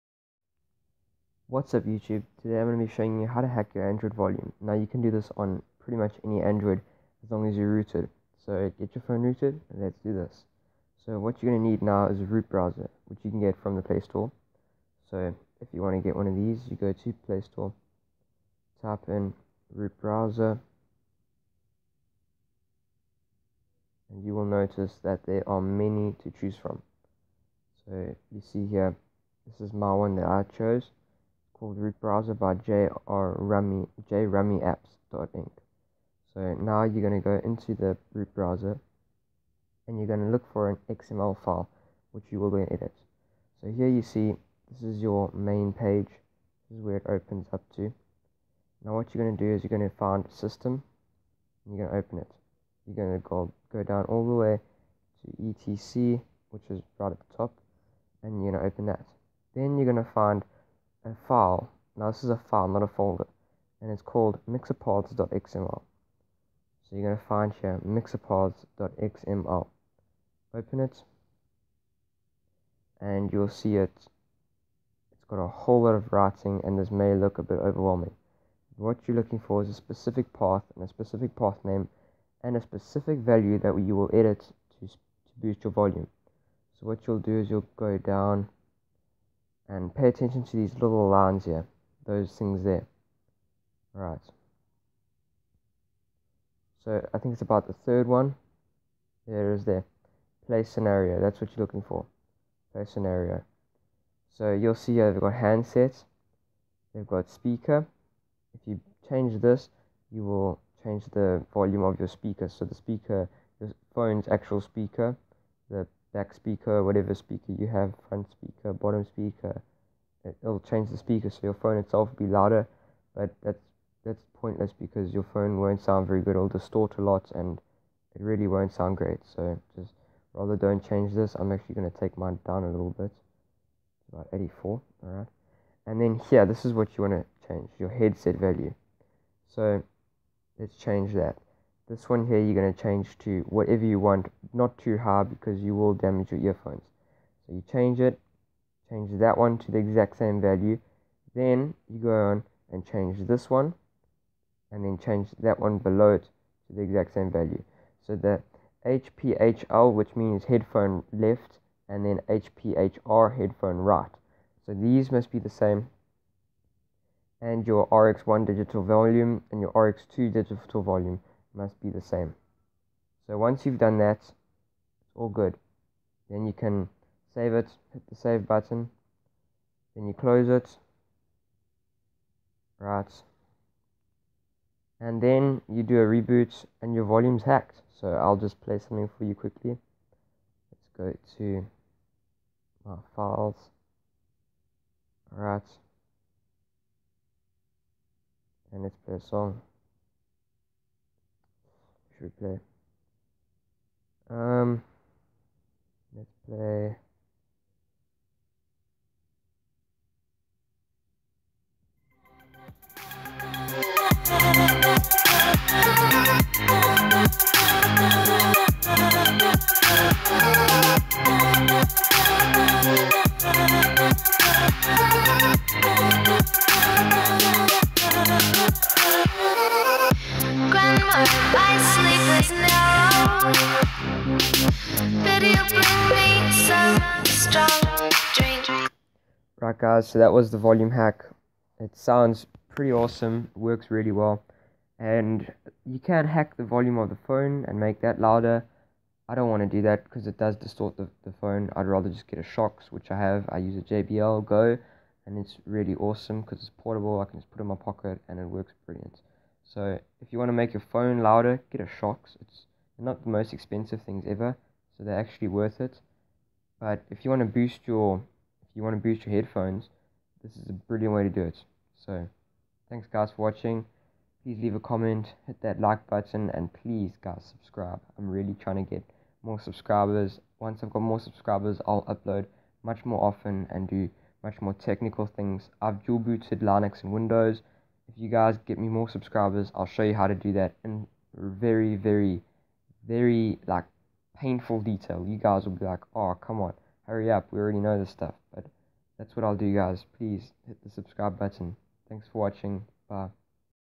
How to Boost your Volume (on any Android)
This is an easy way to boost your androids volume and it works on ANY Android device.

1- Root your phone

3- got to folder root:/system/etc

4- open Mixerpaths.xml

5- find the word string called Play Scenario.

6- Edit the HPHL and HPHR Values and the RX7 Digital Volume values.

7- Save changes

8- Reboot